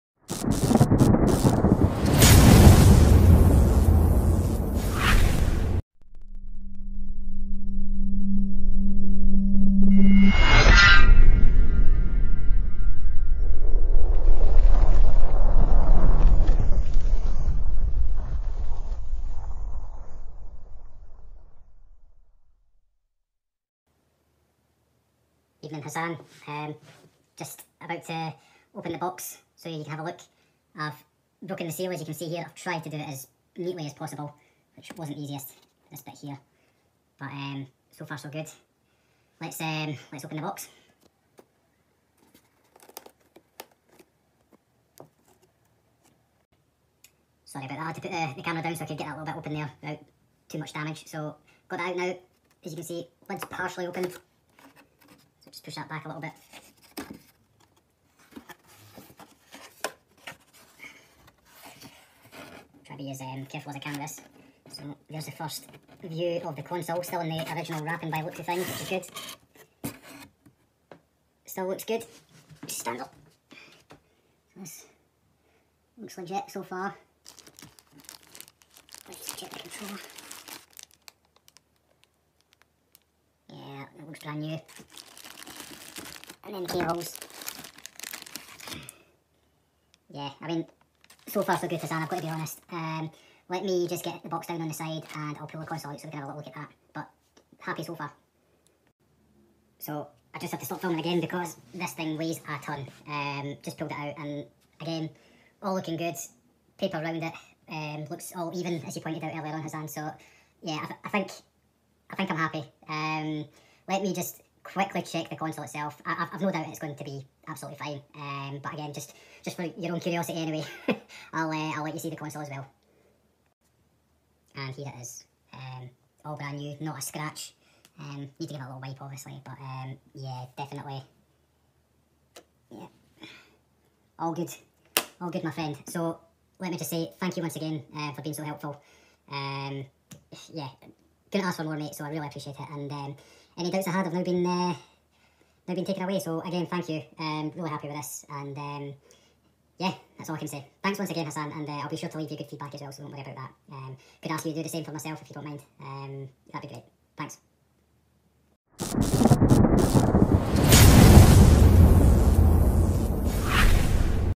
The Last Ever Factory Sealed Sony PS3 60GB Sold and Unboxed by Buyer..
The Last Ever Factory Sealed Sony PlayStation 3 60GB Pal Version sold on Ebay July 13th fit £1,750 and is unboxed by the Buyer..Follow The Shpock Shop on Instragram or on Shpock...